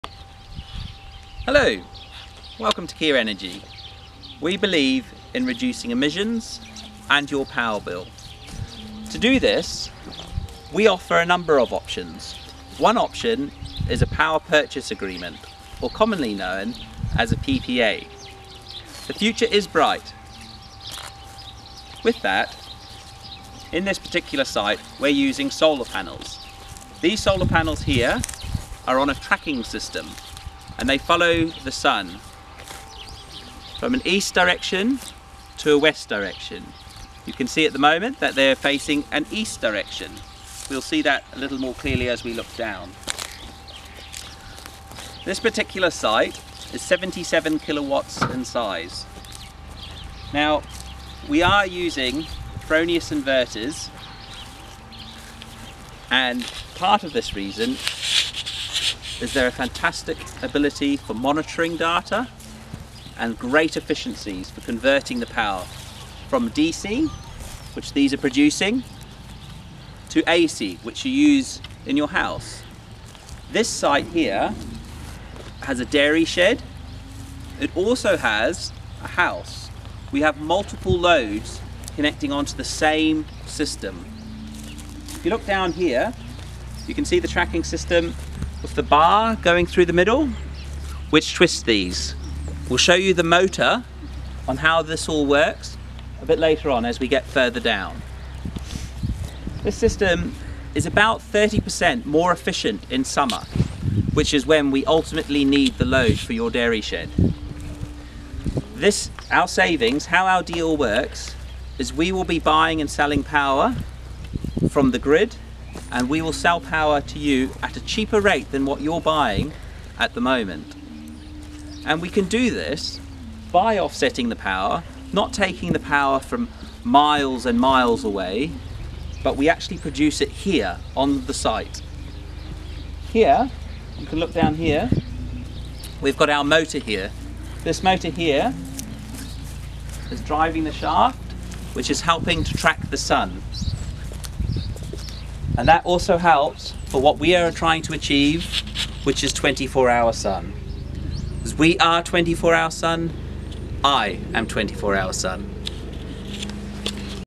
Ground Mount Solar PV tracking system
This site is one of Kea Energy's many ground mount systems. It is a tracking PV system. It is using Kyocera solar panels, located in Canterbury, New Zealand.